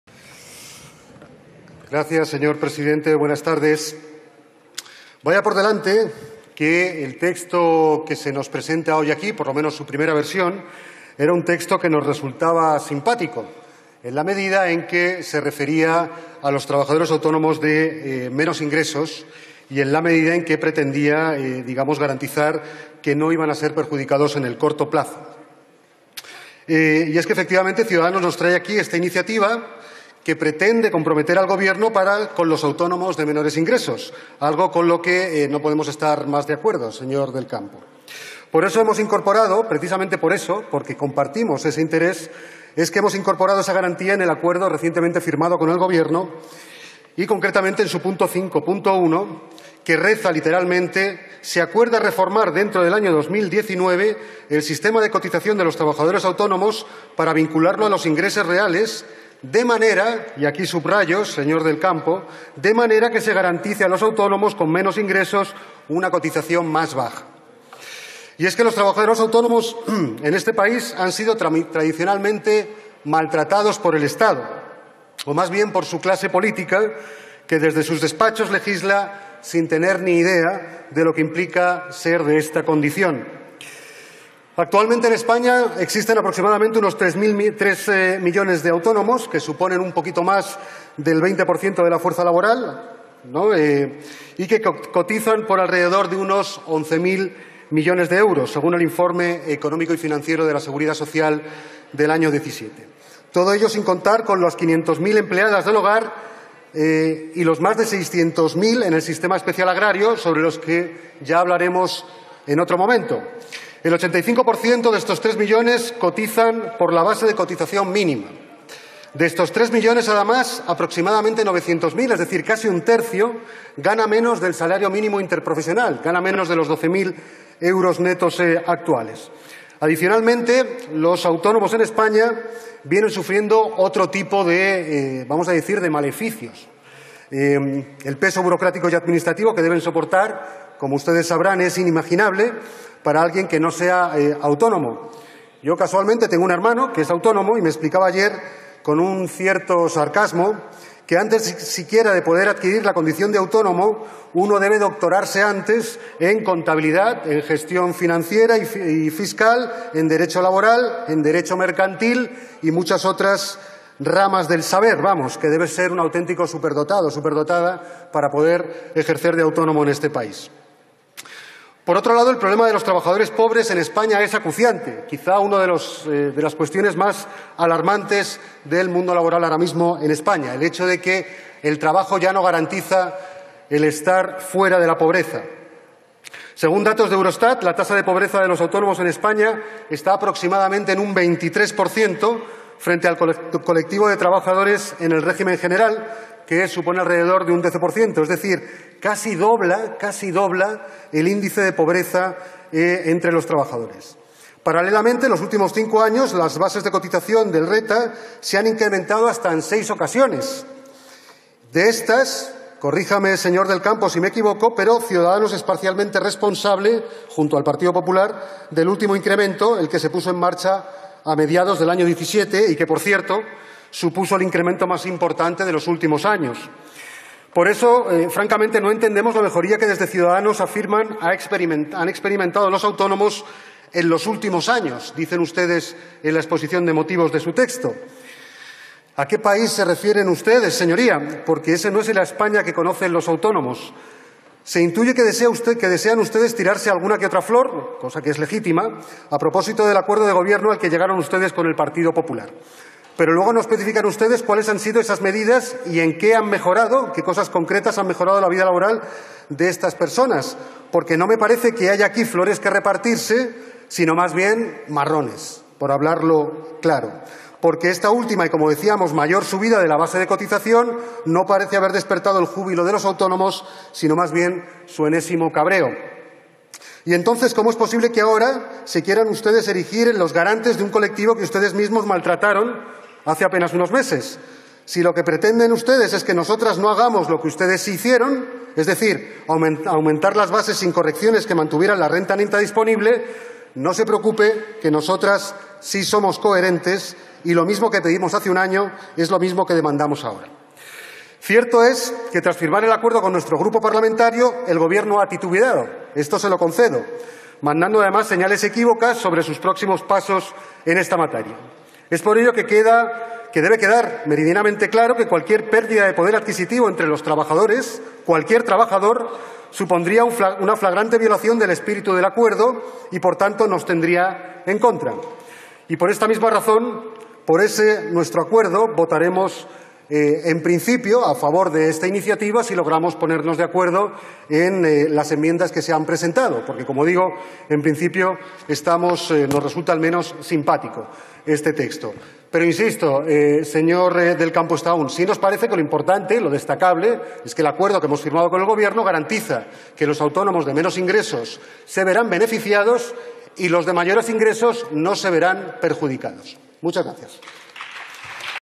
Txema Guijarro sobre las medidas para facilitar la cotización de los autónomos.
Txema Guijarro en sesión plenaria, responde al grupo de Ciudadanos acerca de las medidas para facilitar la cotización de los autónomos. 30 de octubre de 2018